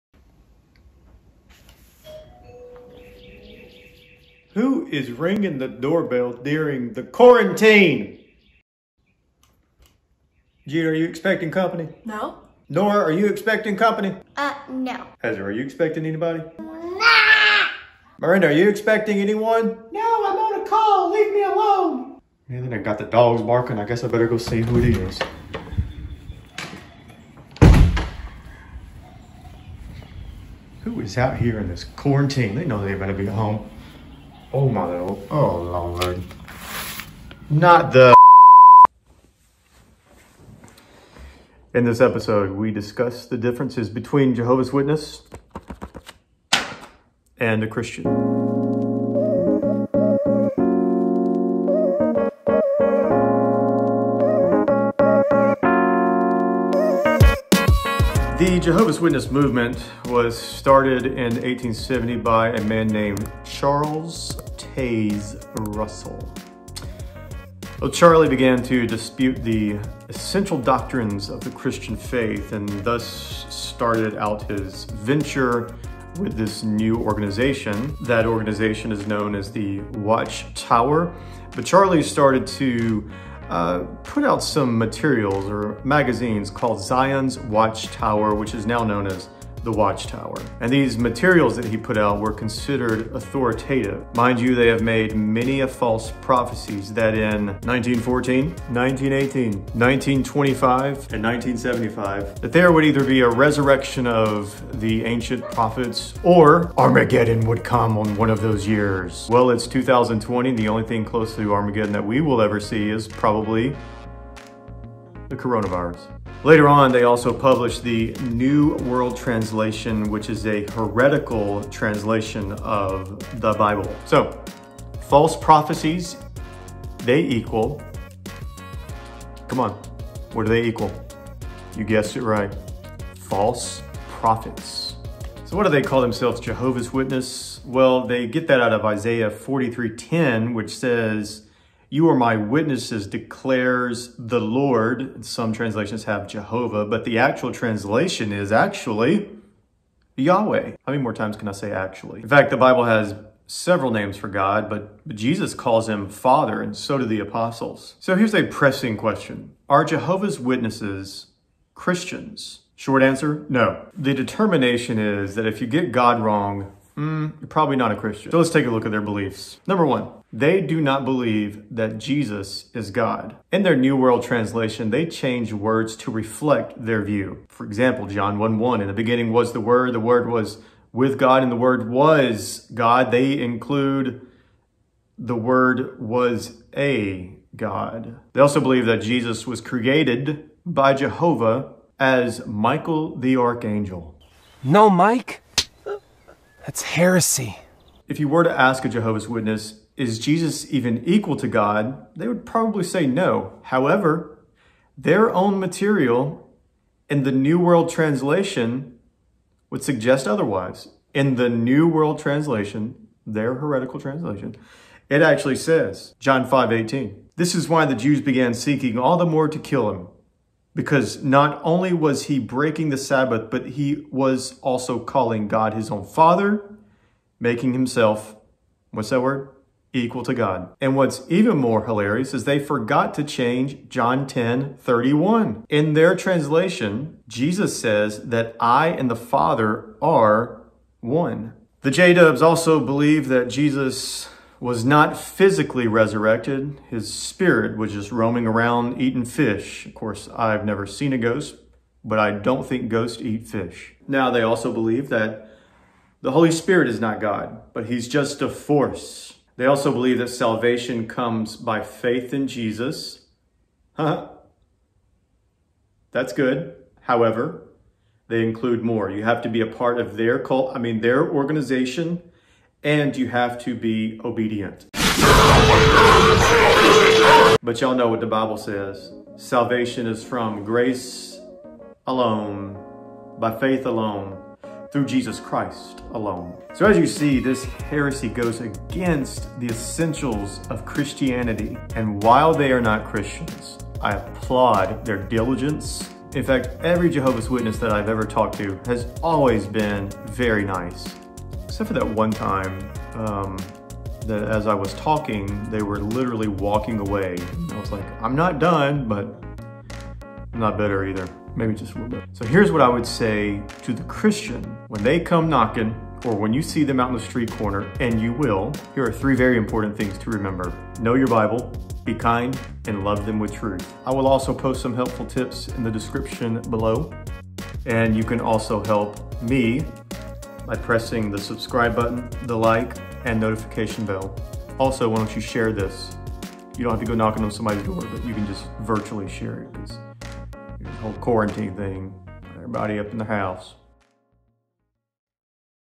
Are Jehovah Witnesses Christians?
In this Episode we talk through the differences of Jehovah Witnesses and Christians. Are Jehovah Witnesses Christians?

Here are more helpful links to help you share the Gospel to Jehovah Witnesses: